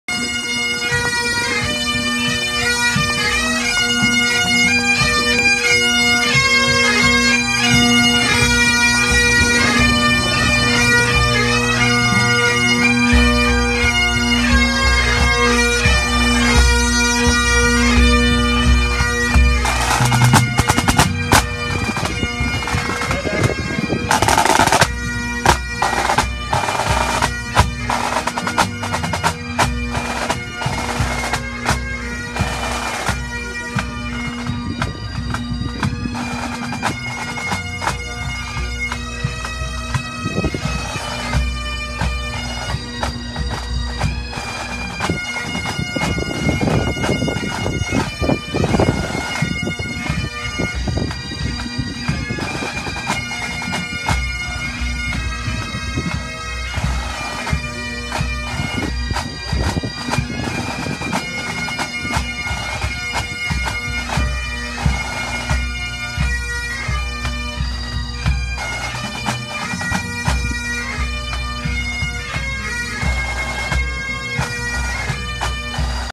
Piper Bobby playing with North Lanarkshire schools pipe band piping live Glasgow August 2016.
This is the band playing in Georges Square, Glasgow. Prior to winning the world championships in their grade.